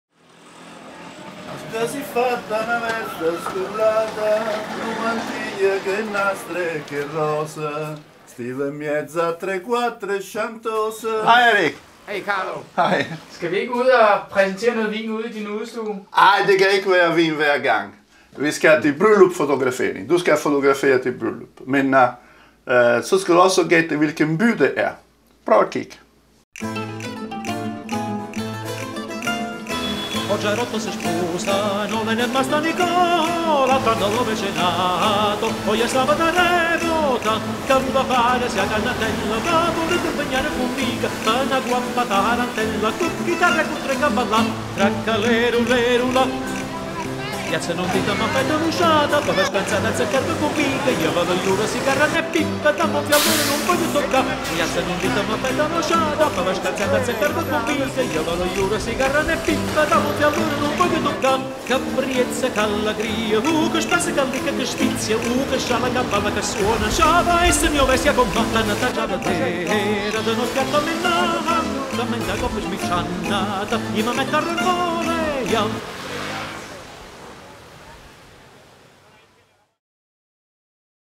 Bryllup i ?
Carlo Merolli besøger en italiensk by. Gæt byen og skriv til Carlo.